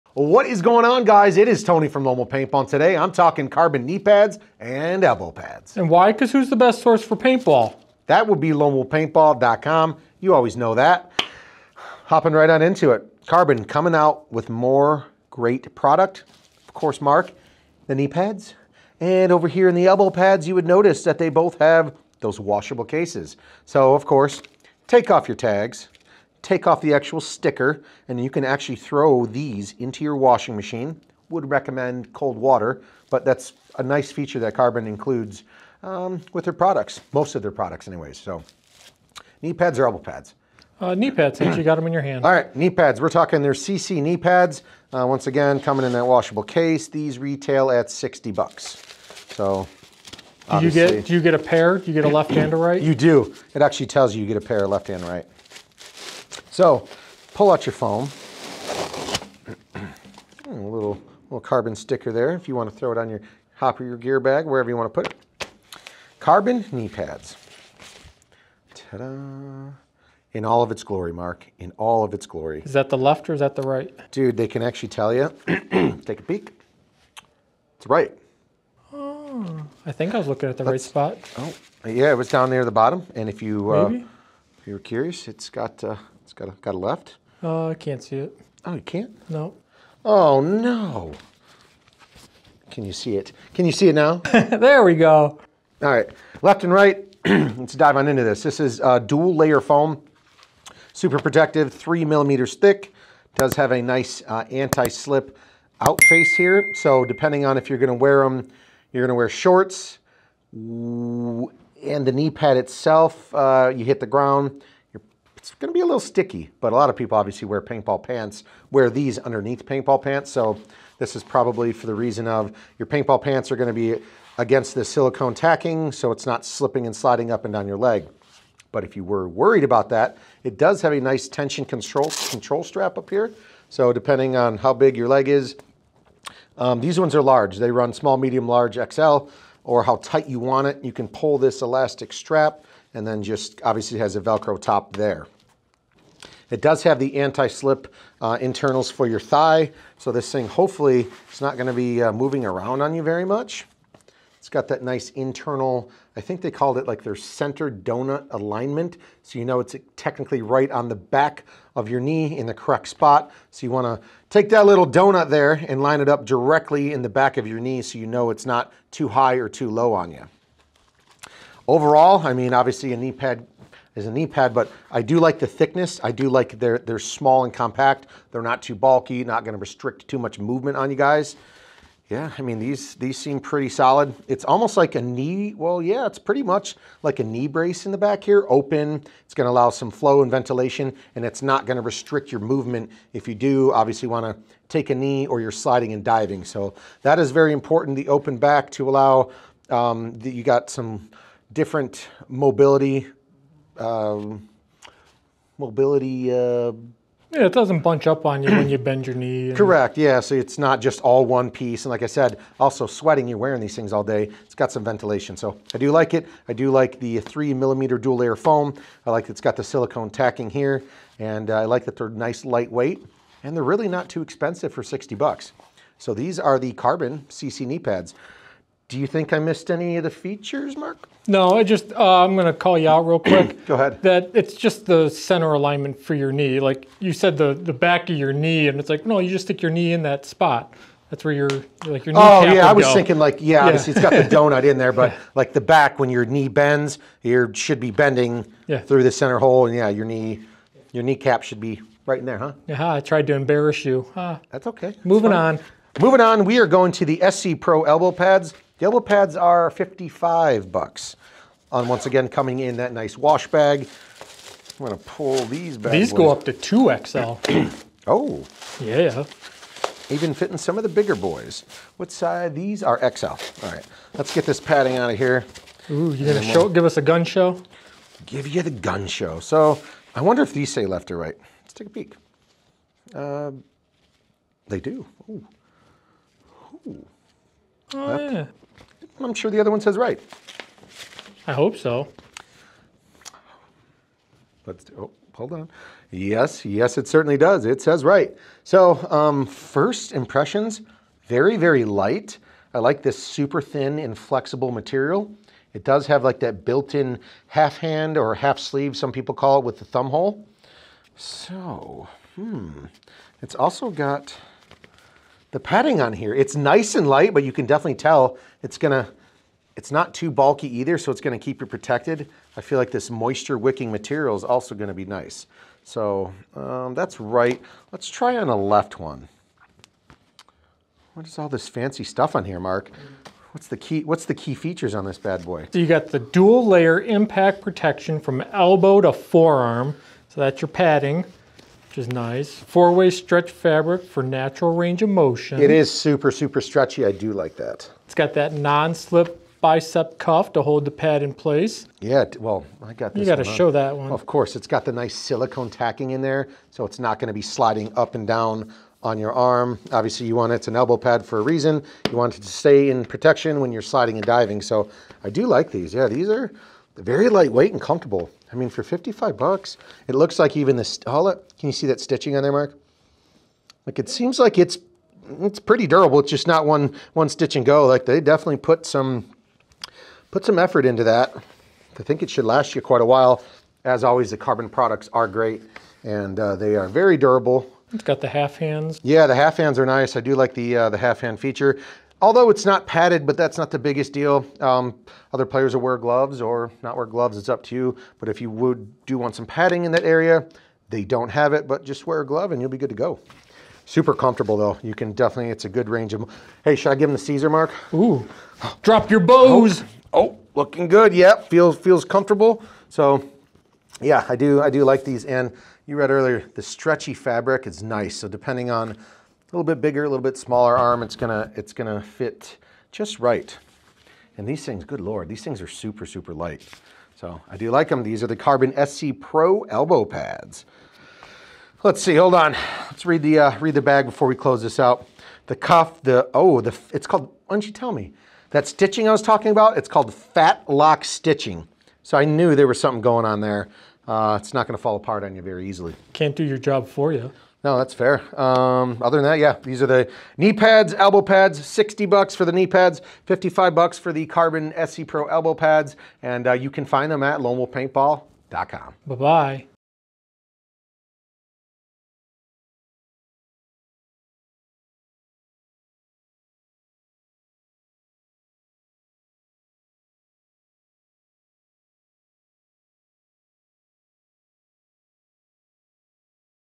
Carbon SC Elbow Sleeves and Carbon CC Knee Pads Overview | Lone Wolf Paintball
Carbon SC Elbow Sleeves and Carbon CC Knee Pads Overview
Tony shares the features of the Carbon SC Elbow Pads and Carbon CC Knee Pads.

00:00 Intro
00:16 Washable Cases
00:42 Carbon CC Knee Pads
04:16 Carbon SC Elbow Pads
07:28 Closing